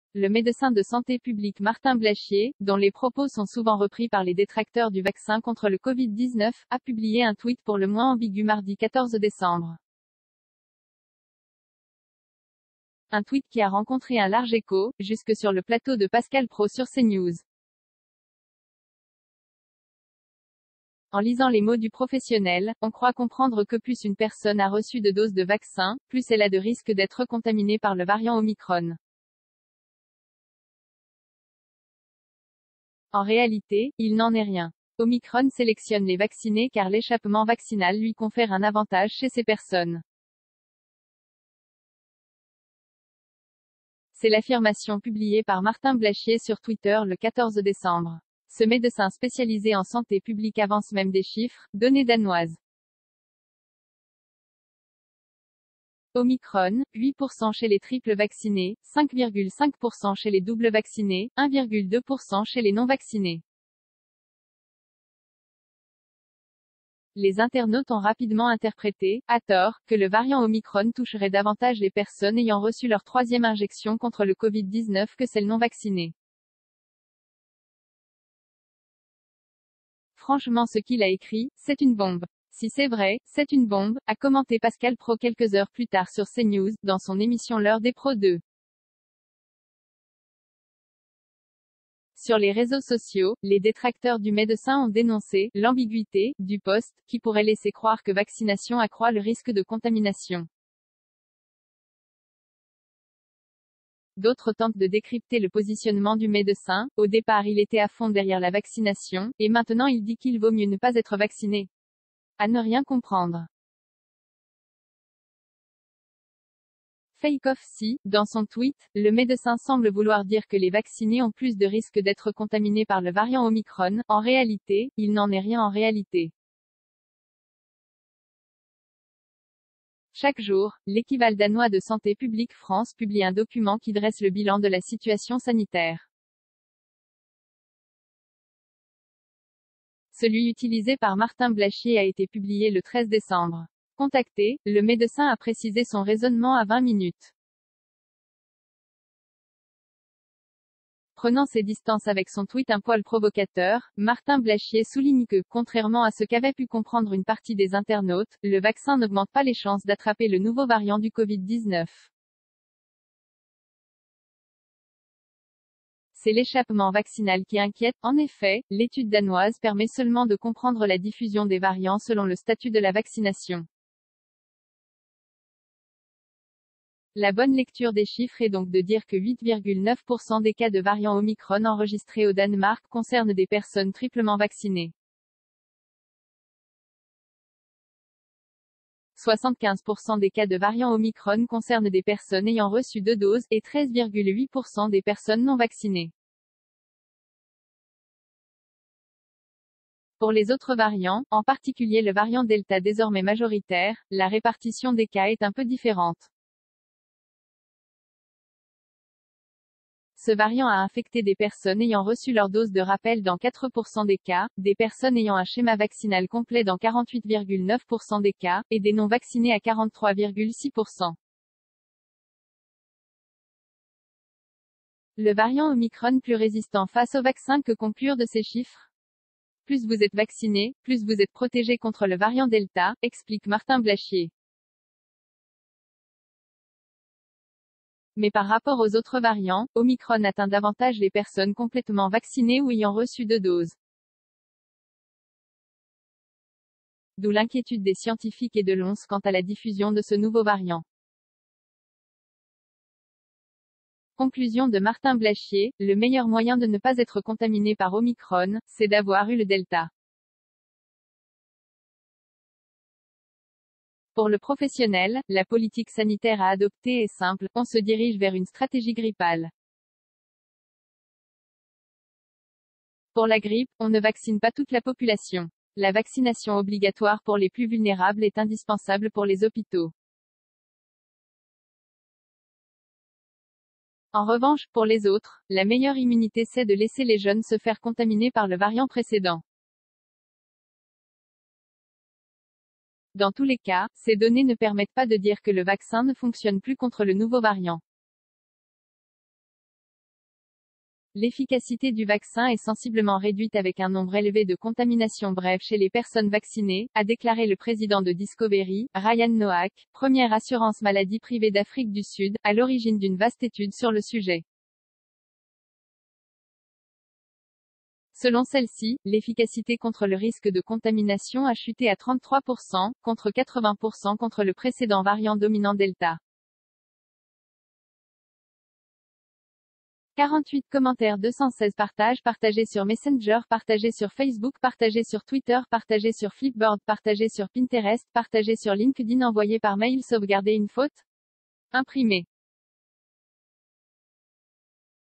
Coronavirus : Les personnes vaccinées ont-elles plus de risques d'attraper le variant Omicron ? Non
Le médecin de santé publique Martin Blachier, dont les propos sont souvent repris par les détracteurs du vaccin contre le Covid-19, a publié un tweet pour le moins ambigu mardi 14 décembre. Un tweet qui a rencontré un large écho, jusque sur le plateau de Pascal Praud sur CNews.   En lisant les mots du professionnel, on croit comprendre que plus une personne a reçu de doses de vaccin, plus elle a de risques d’être contaminée par le variant Omicron.   En réalité, il n’en est rien.    « Omicron sélectionne les vaccinés car l’échappement   vaccinal lui confère un avantage chez ces personnes. » C’est l’affirmation publiée par Martin Blachier sur   Twitter le 14 décembre. Ce médecin spécialisé en santé publique avance même des chiffres : « Données danoises. Omicron : 8 % chez les triples   vaccinés, 5,5 % chez les doubles vaccinés, 1,2 % chez les non-vaccinés. »   Les internautes ont rapidement interprété, à tort, que le variant Omicron toucherait davantage les personnes ayant reçu leur troisième injection contre le Covid-19 que celles non vaccinées. « Franchement ce qu’il a écrit, c’est une bombe ! Si c’est vrai, c’est une bombe ! », a commenté Pascal Praud quelques heures plus tard sur CNews, dans son émission L’Heure des Pros 2.   D’autres tentent de décrypter le positionnement du médecin : « Au départ il était à fond derrière la vaccination, et maintenant il dit qu’il vaut mieux ne pas être vacciné ? A ne rien comprendre. »   FAKE OFF   Si, dans son tweet, le médecin semble vouloir dire que les vaccinés ont plus de risque d’être contaminé par le variant Omicron, en réalité, il n’en est rien en réalité. Chaque jour, l'équivalent danois de Santé publique France publie un document qui dresse le bilan de la situation sanitaire. Celui utilisé par Martin Blachier a été   publié le 13 décembre.    Contacté, le médecin a précisé son raisonnement à 20 Minutes. Prenant ses distances avec son tweet un poil provocateur, Martin Blachier souligne que, contrairement à ce qu’avaient pu comprendre une partie des internautes, « le vaccin n’augmente pas les chances d’attraper le nouveau variant du Covid-19 ». « C’est l’échappement vaccinal qui inquiète. »   En effet, l’étude danoise permet seulement de comprendre la diffusion des variants selon le statut de la vaccination. La bonne lecture des chiffres est donc de dire que 8,9 % des cas de variant Omicron enregistrés au Danemark concerne des personnes triplement vaccinées. 75 % des cas de variant Omicron concernent des personnes ayant reçu deux doses, et 13,8 % des personnes non-vaccinées.   Pour les autres variants, en particulier le variant Delta désormais majoritaire, la répartition des cas est un peu différente. Ce variant a infecté des personnes ayant reçu leur dose de rappel dans 4 % des cas, des personnes ayant un schéma vaccinal complet dans 4